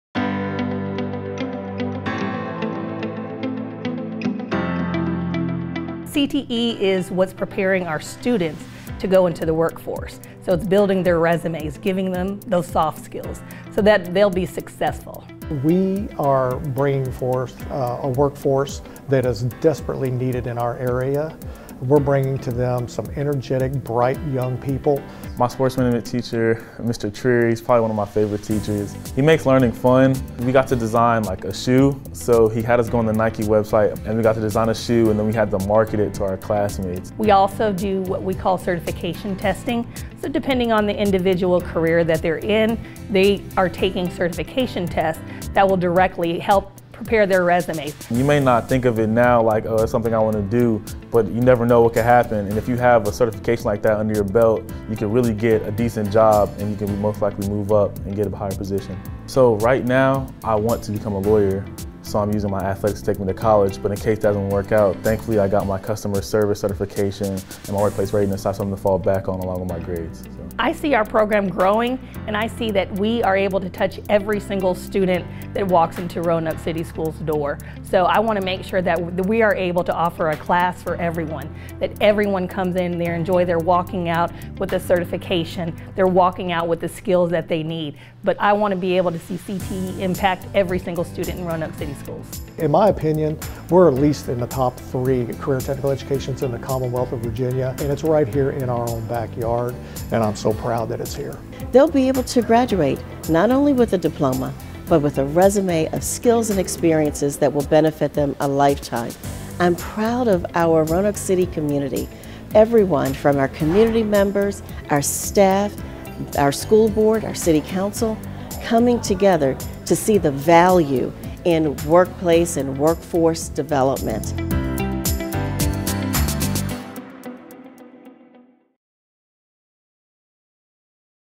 Diploma and a Resume: Career & Technical Education in Roanoke City Public Schools
February is Career & Technical Education (CTE) month. Take a moment to learn how RCPS is providing students with hands-on experiences so they can graduate with a diploma and a resume. With the recent opening of the Charles W. Day Technical Education Center (DAYTEC), we're continuing to expand so every student can find a class they're interested in.